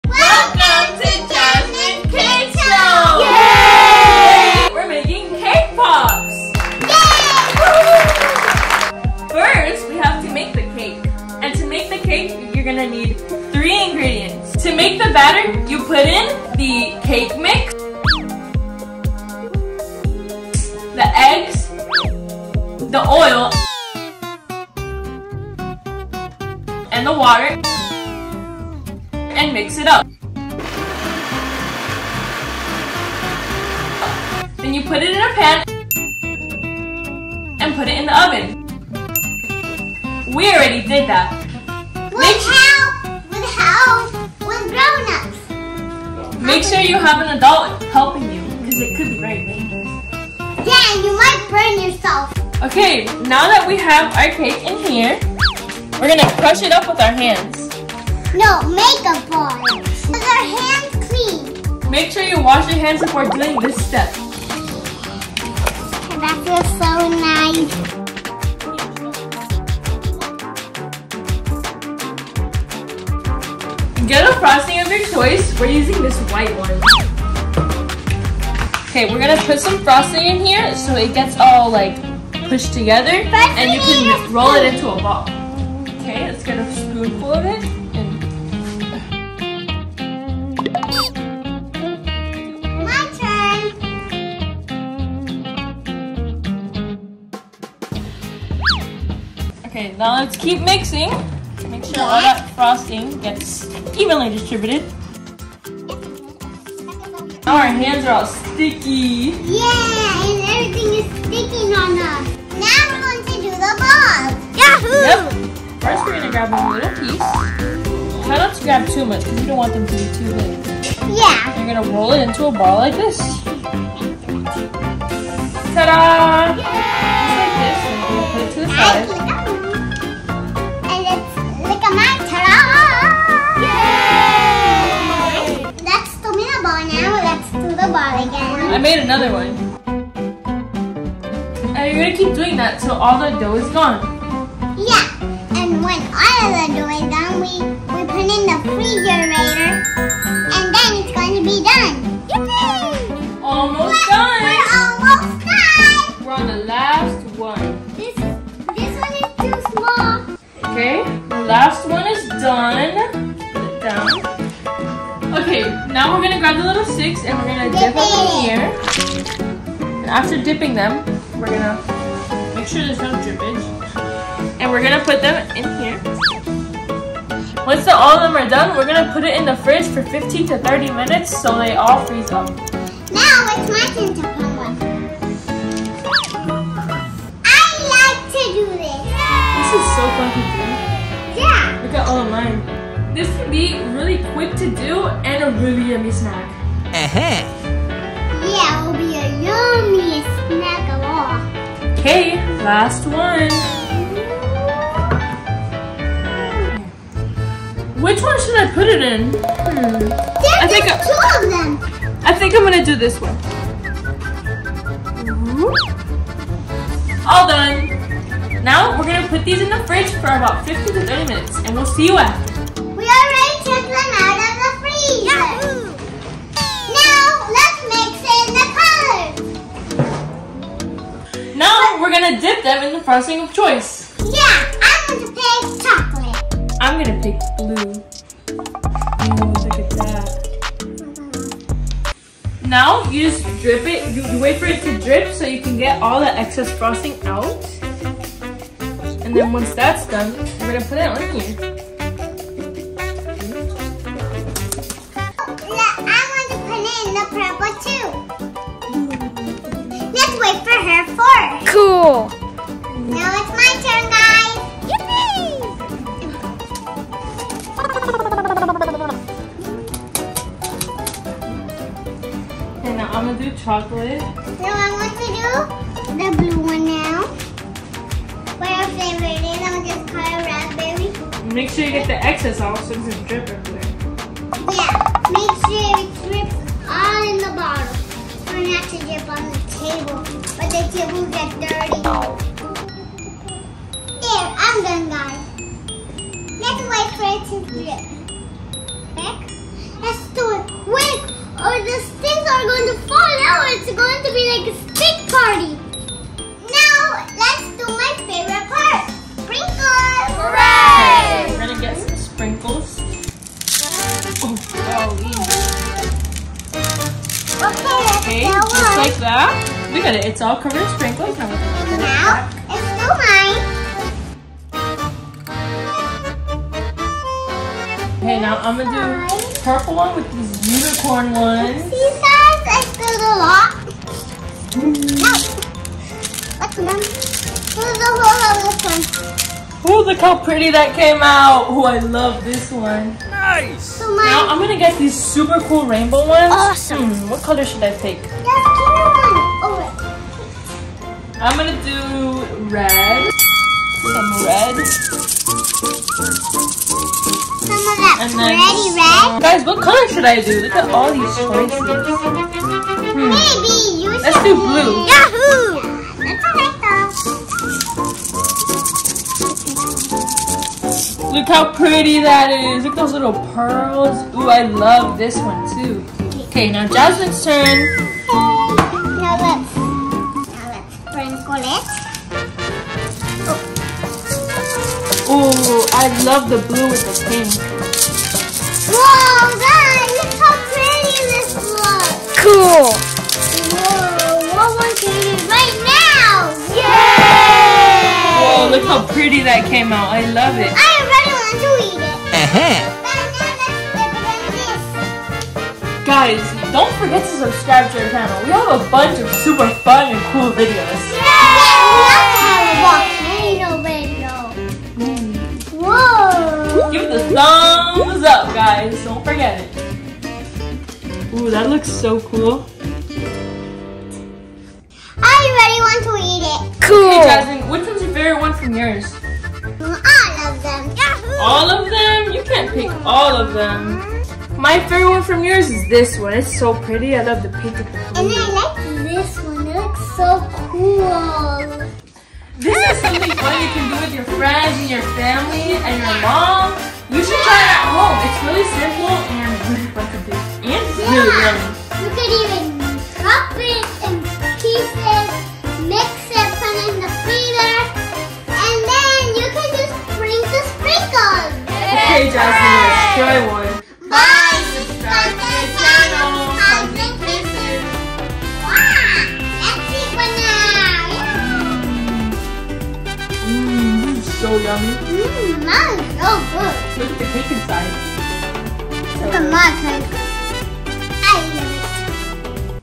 How To Make Cake Pops With Jasmin! | Jasmin Kids Show
Learn how to make cake pops with Jasmin with this step by step tutorial! She explains how to make them from scratch!